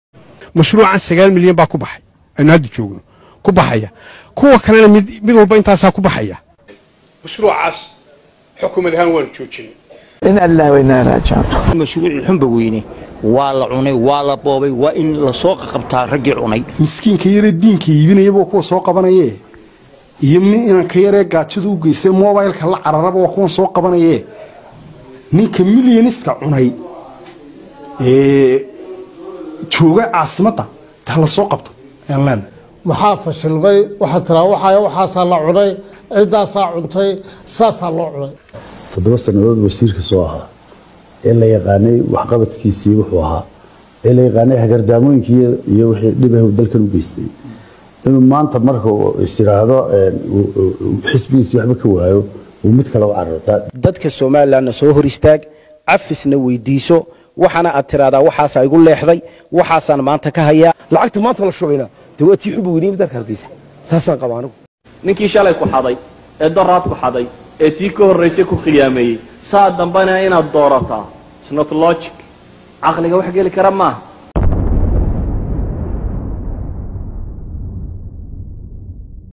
Yaa cunay lacagtii Mashruucii Xumba-weyne?
Maxamuud Xaashi (KAAH) wuxuu lunsadey in ka badan 7 milyan oo Doolar oo ahayd lacagtii loogu tala galay Mashruucii Xumba-Weyne ee biyaha Hargeysa.
Maantana Maxamuud Xaashi wuxuu u socdaa sidii uu markale mashaariic kale u lunsan lahaa.